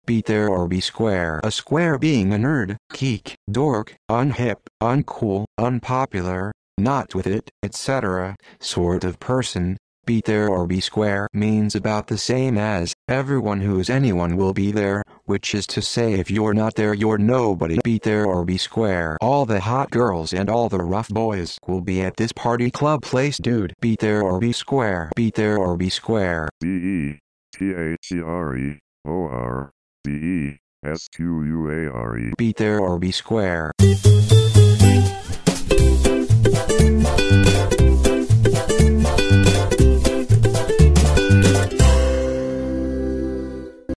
be there or be square meaning and pronunciation
A square being a nerd, geek, dork...un-hip, un-cool, unpopular, not with-it, etc. ...sort of person..."be there or be square" means about the same as "everyone who is anyone will be there", which is to say if you're not there you're nobody.

be there or be square definition by Urban Dictionary